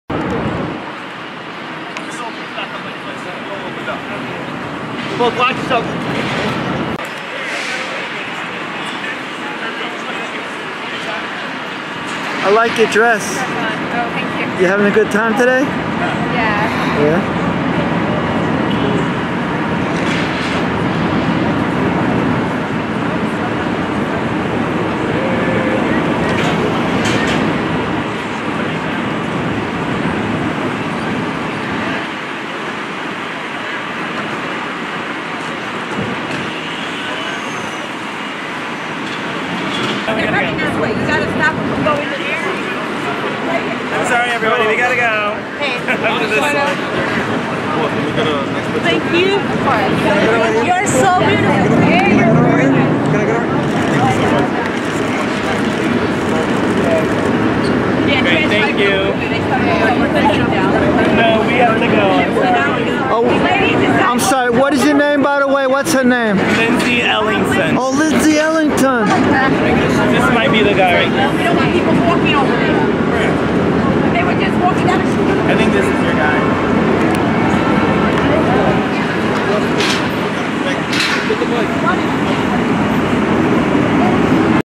Lindsay Ellingson attended "Glamour"awards event
Lindsay  Ellingson outside Carnegie Hall after appearing on the Red Carpet at the Glamour Awards 2015.